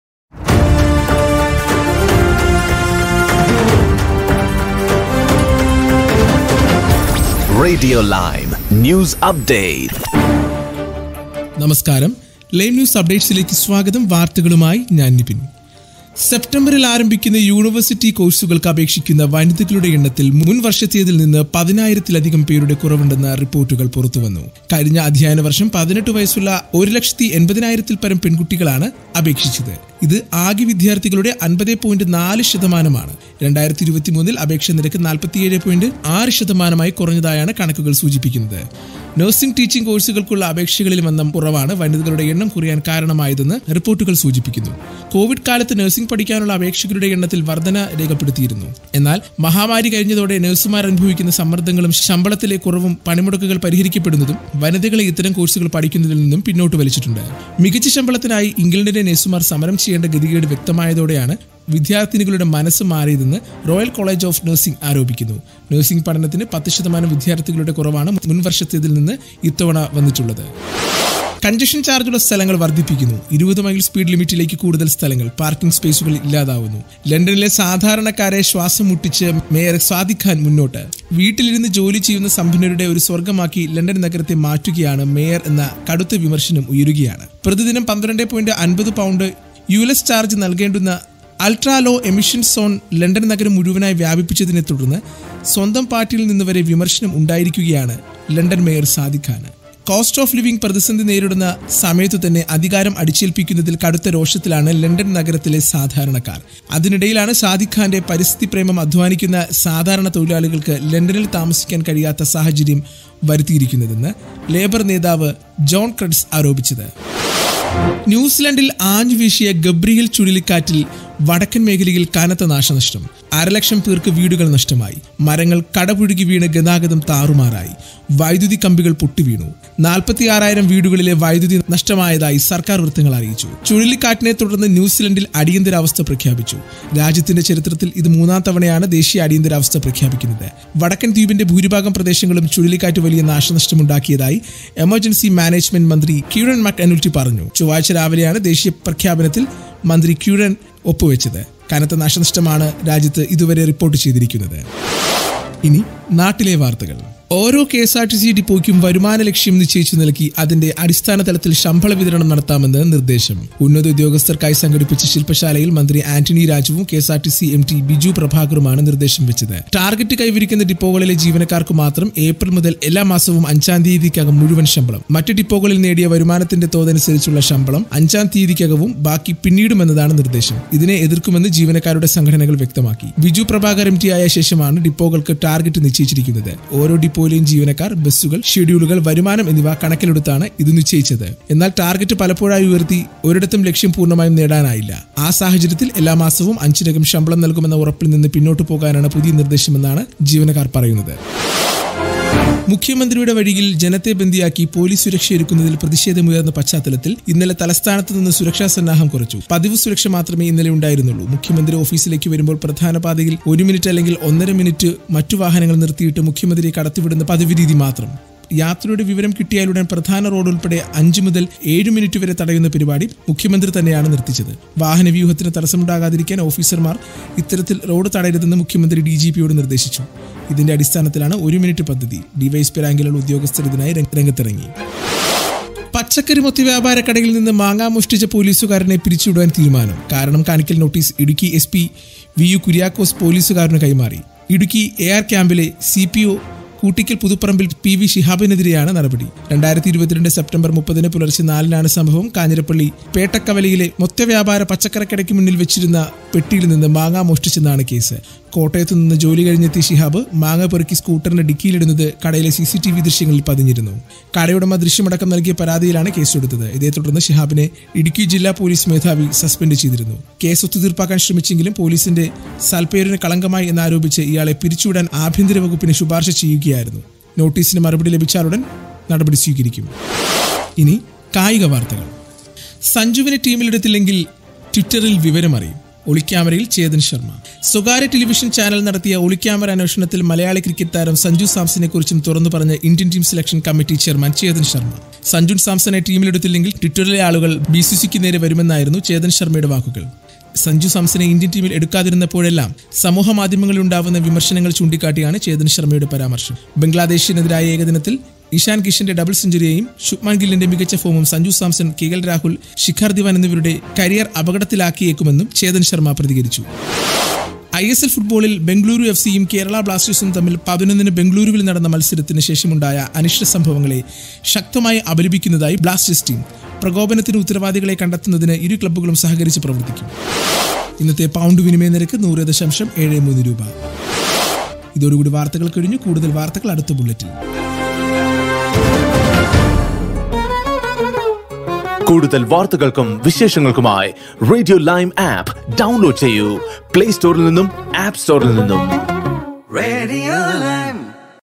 കെഎസ്ആർടിസി: ടാർഗറ്റ് തികച്ചാൽ 5ന് പൂർണ ശമ്പളം; നിർദേശവുമായി മന്ത്രിയും എംഡിയും|NEWS UPDATE|RJ NIBIN
Tune into Radio Lime- The First South Indian Multilingual Digital Radio Station in the UK.
How to listen to Radio